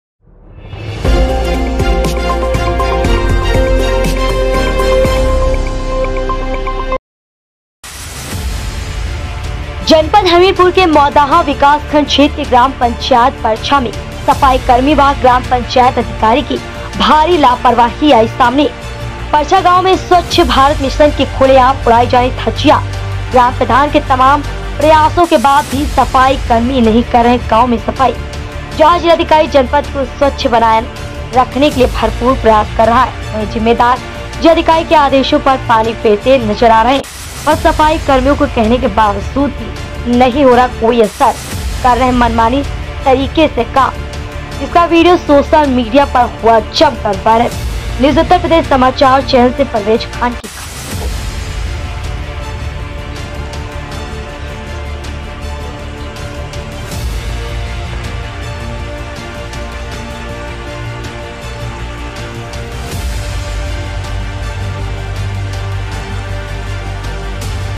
परछा गांव में स्वच्छ भारत मिशन की खुले आम उड़ाई जा रही धज्जियां...
मौदहा विकासखंड क्षेत्र के ग्राम पंचायत परछा में सफाई कर्मी व ग्राम पंचायत अधिकारी की भारी लापरवाही आई सामने